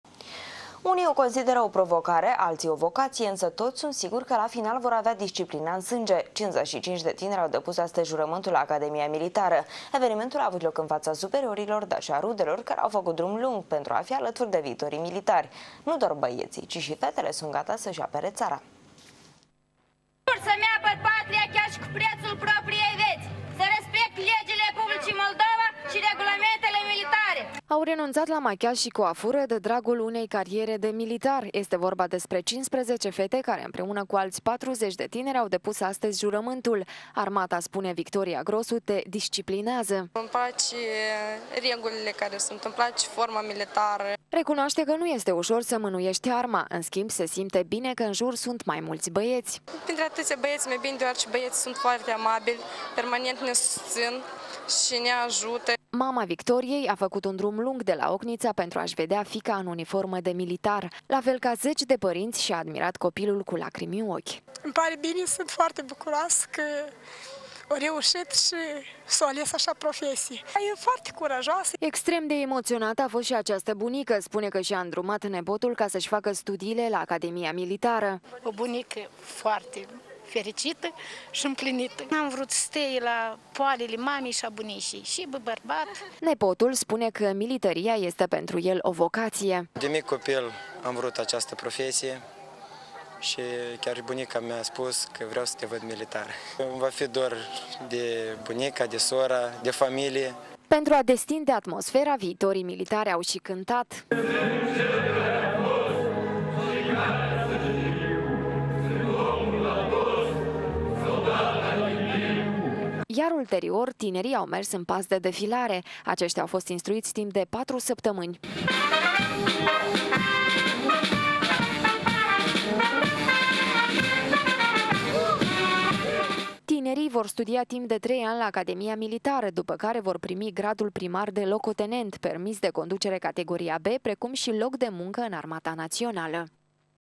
55 de tineri au depus juramantul la Academia Militara - Pro Tv Chisinau
Unii o considera o provocare, altii - o vocatie, insa toti sunt siguri ca la final vor avea disciplina in sange. 55 de tineri au depus juramantul la Academia Militara